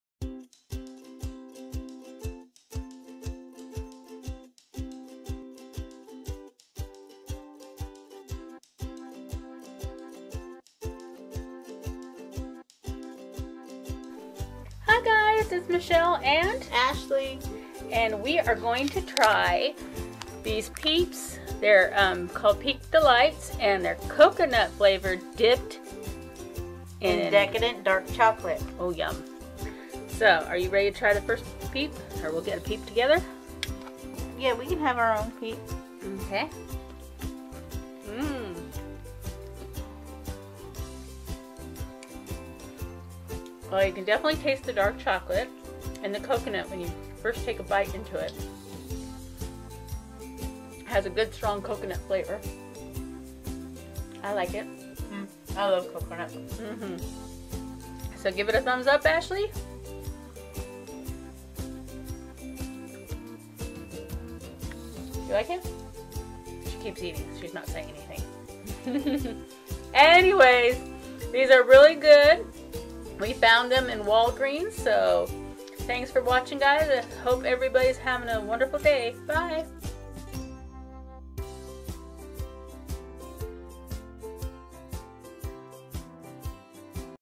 Taste test of Peeps Delights Coconut flavored marshmallow dipped in decadent dark chocolate
Found this yummy new flavor of Peeps at Walgreens! if you love dark chocolate and coconut, you are gonna love these!